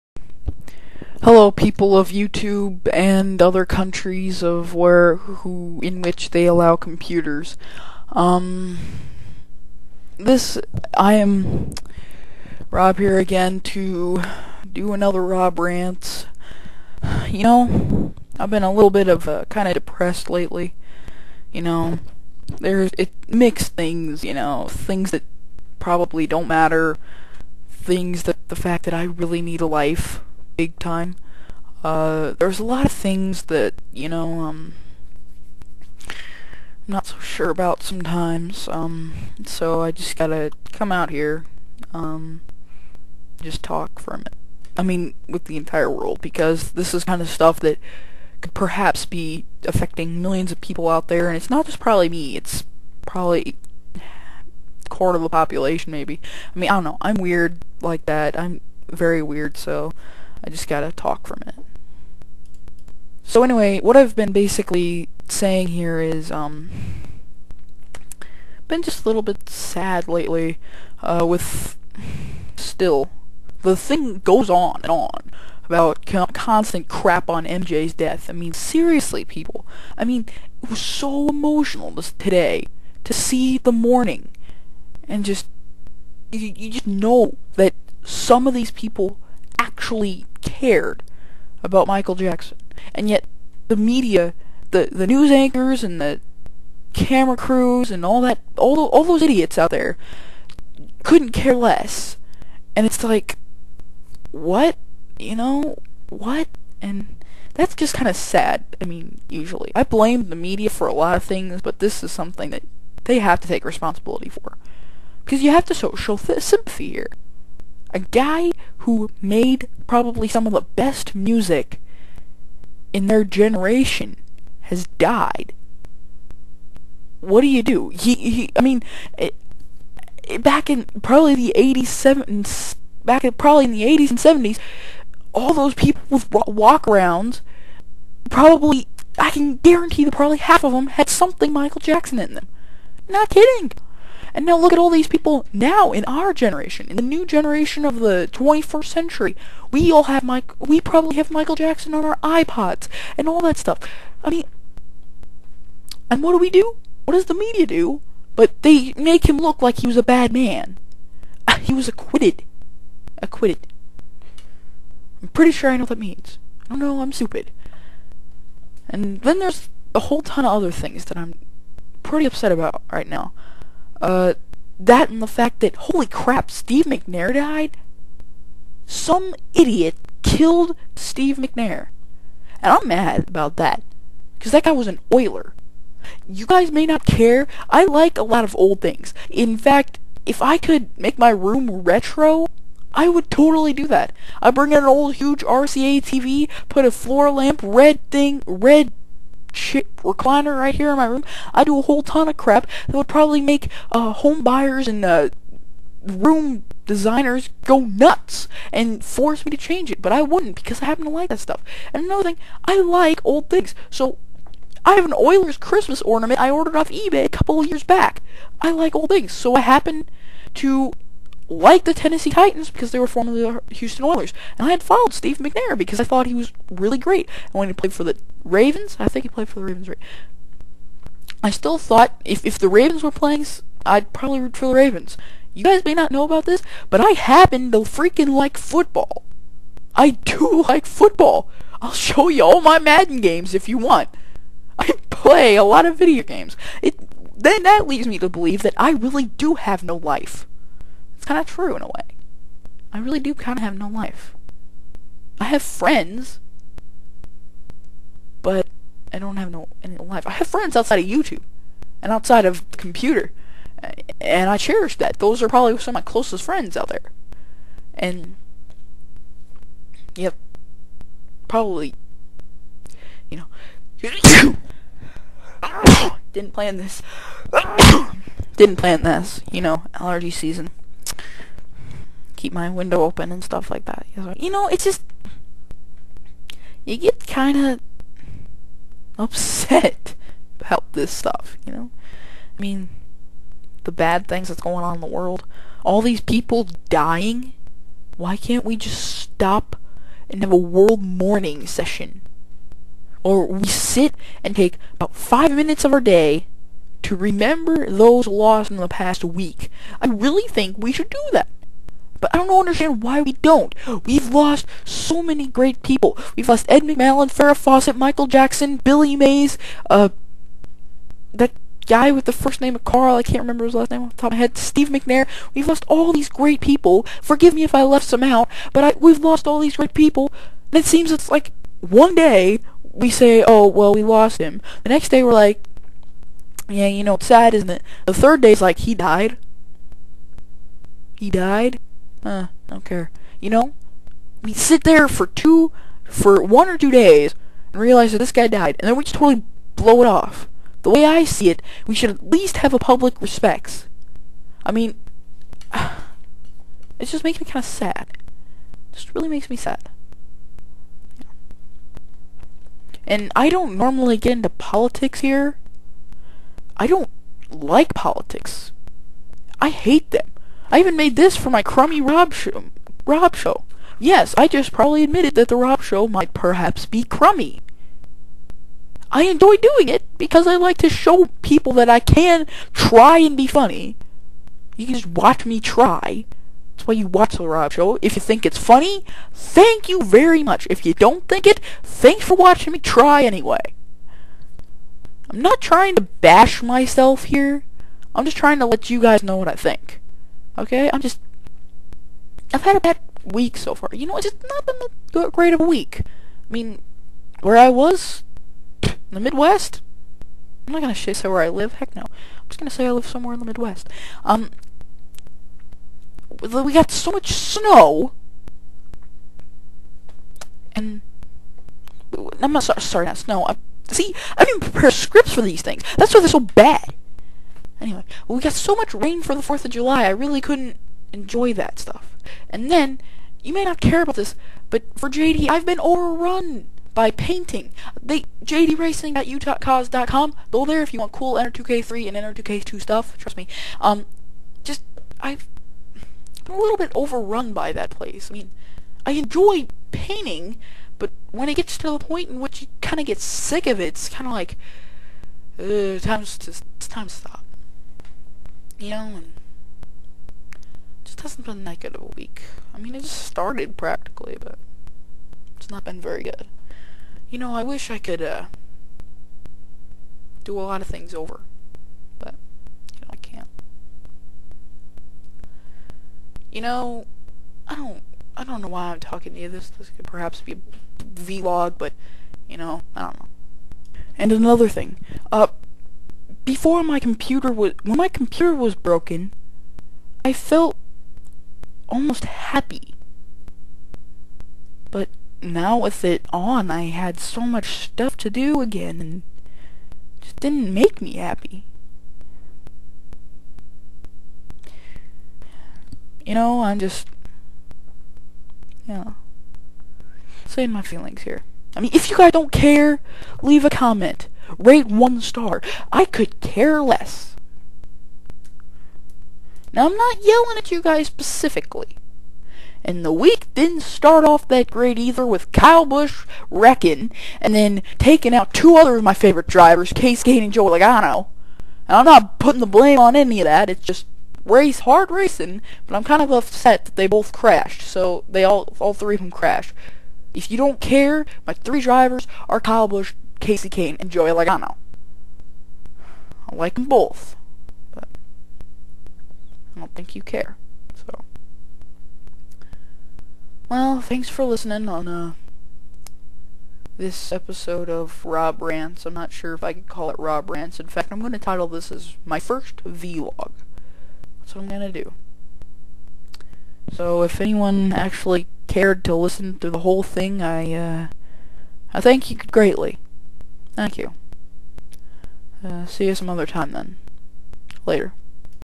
My First VLog
What started out as a Rob Rants turned into a VLog. Enjoy. I'm sad in this one if anyone cares.